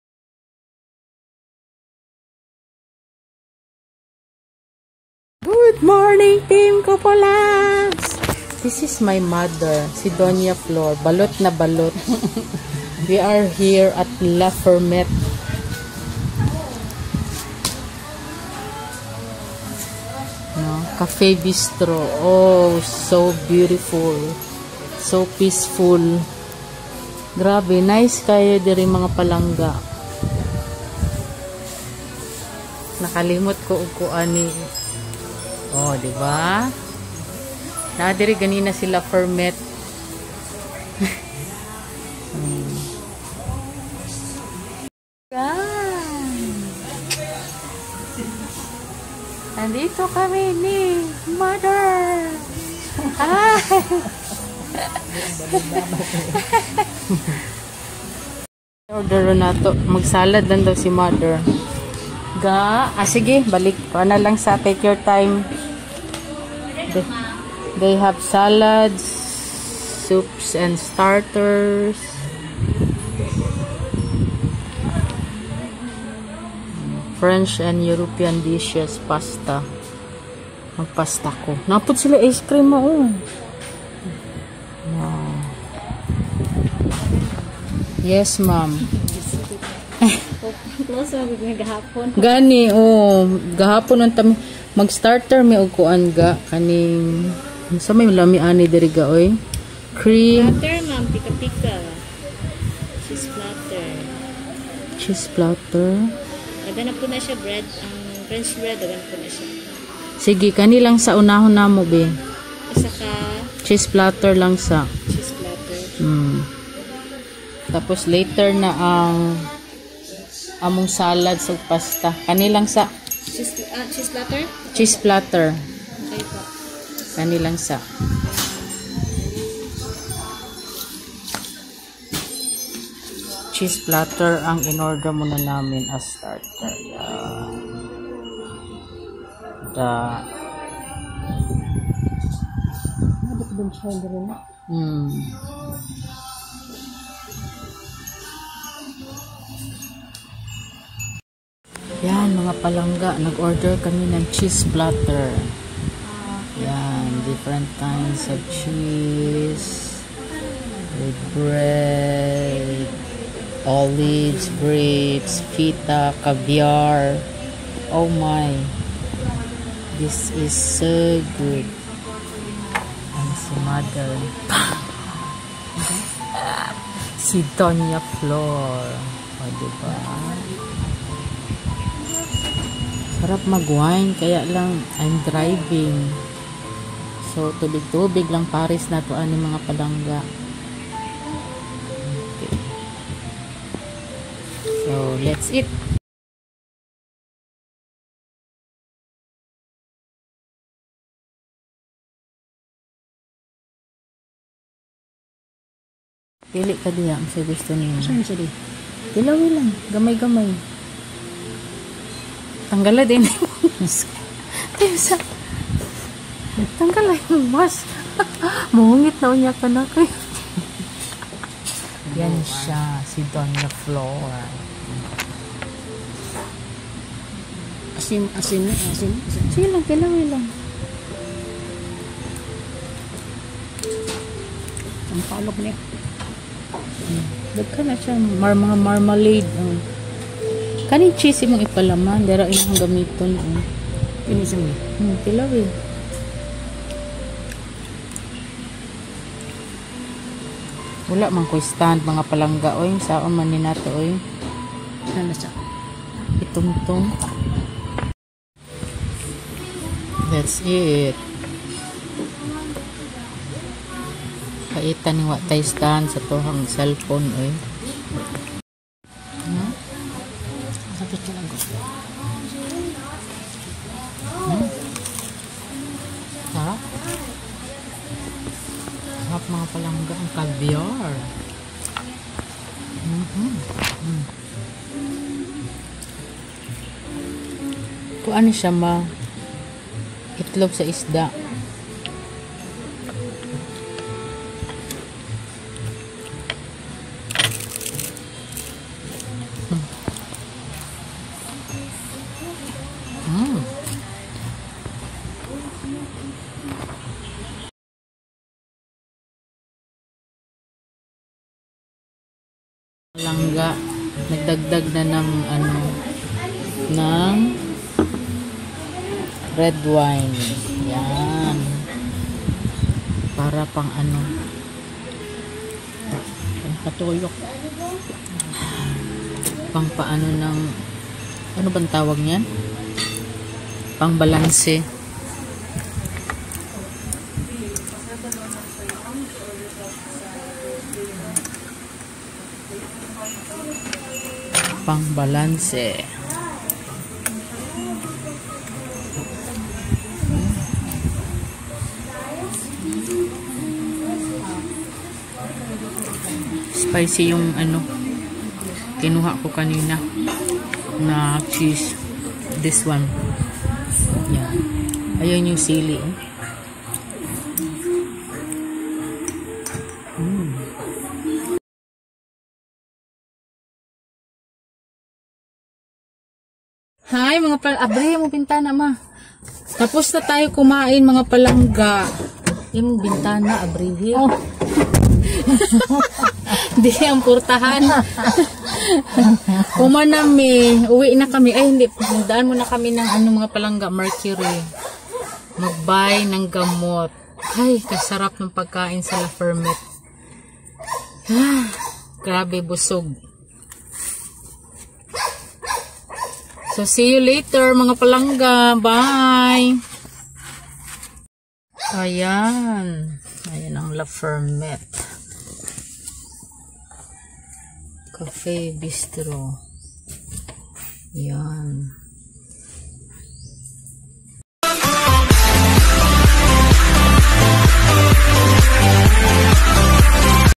One Fine Day at the farm | French style
My Mama (Donya Flor) and I had a great time spending it at the farm. We met in person with my new found friend La Fermette (Eric). The place is so nice and clean. Especially the food, it really tastes nice.

I missed documenting some of the moments and the food we ordered. Kay na overwhelmed sa kalami sa food.  By the way thank you La Fermette for the Yogurt and breadfruits.